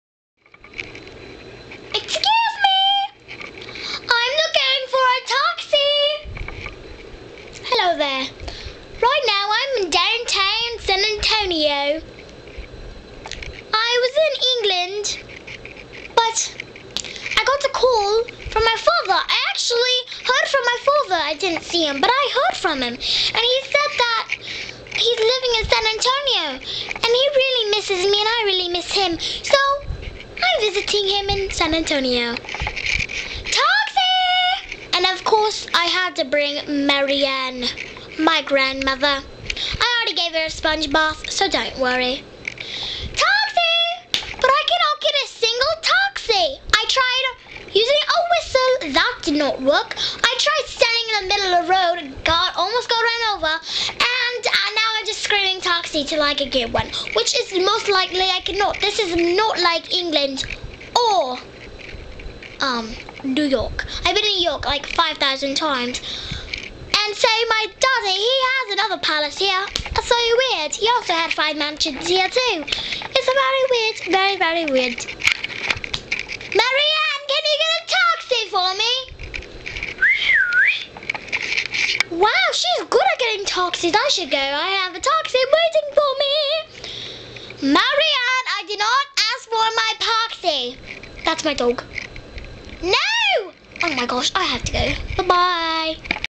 Where's Kelby (Act 1)?
My little sister went missing for a few hours in our home and this is what was found. Kelby was 9 when she recorded this in her room. Kids do the funniest things! Make sure you see the follow up!